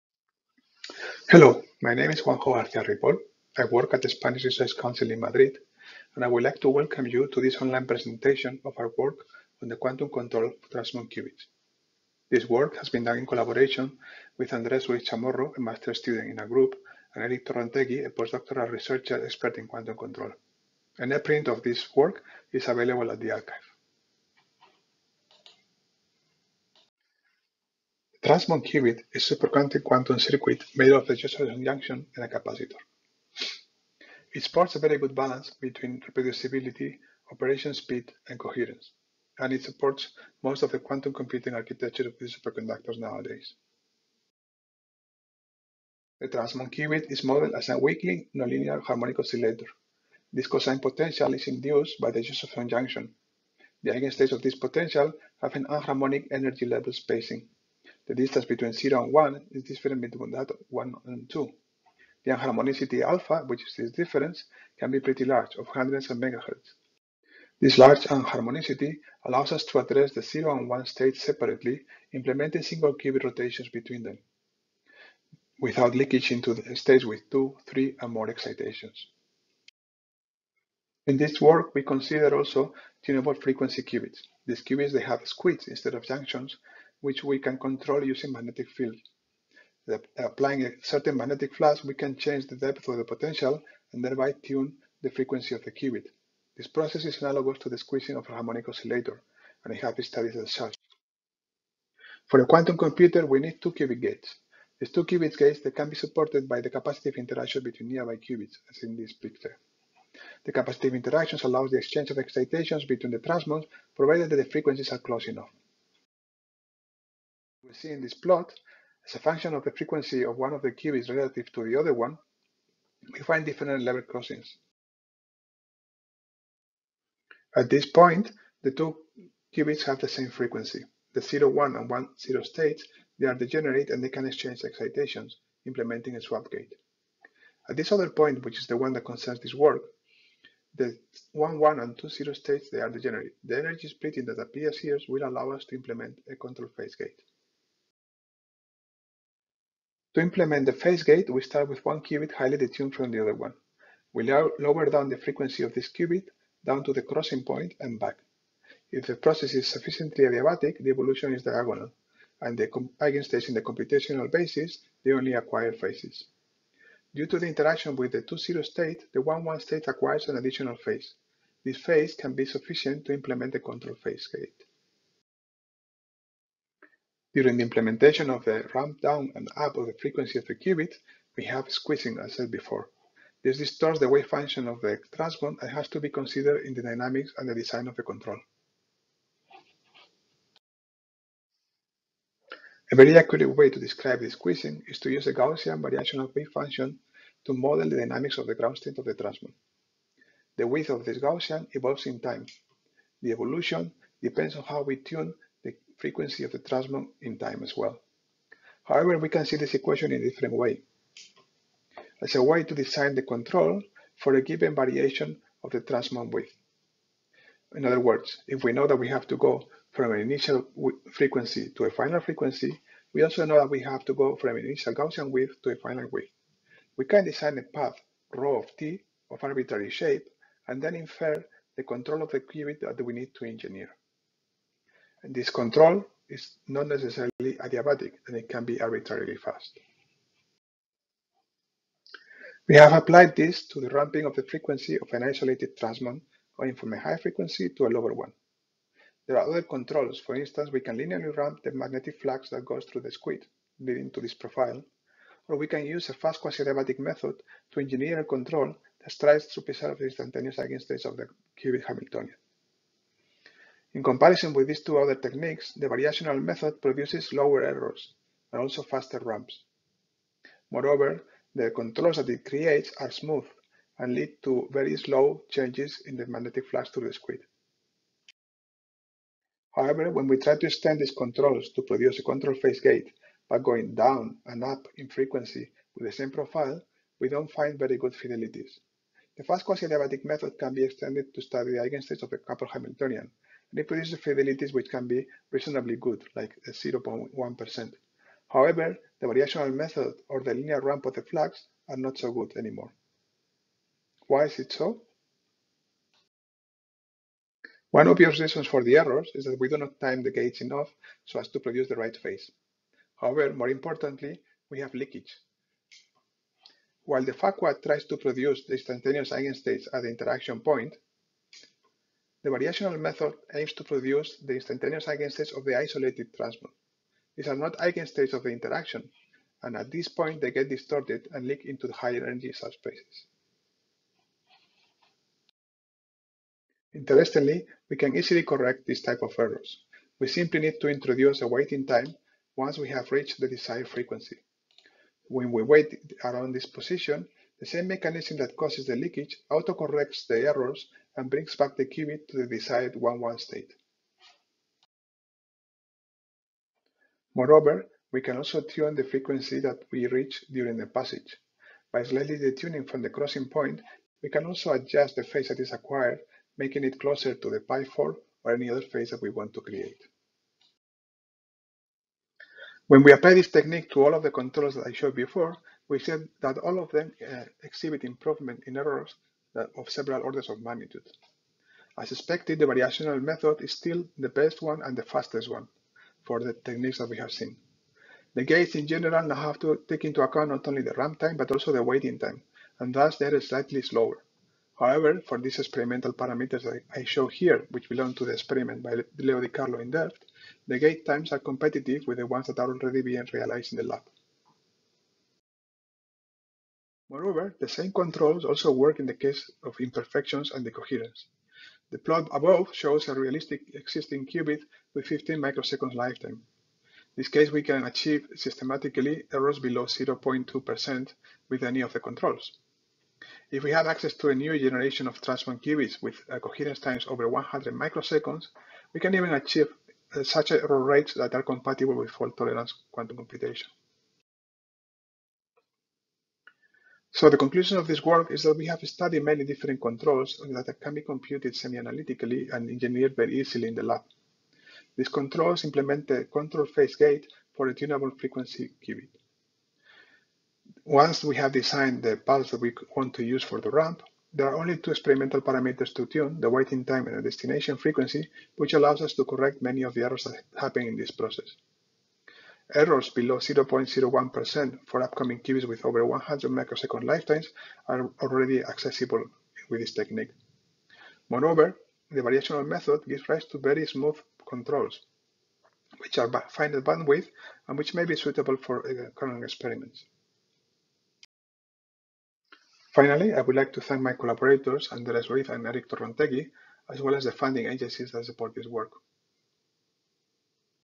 Variational quantum control for single- and two-qubit transmon gates
This video contains the talk that I would have given at the Optimal Quantum Control session of the APS March Meeting 2020, in Denver, linked

Apologies for the voice. I was sick at the time of this recording.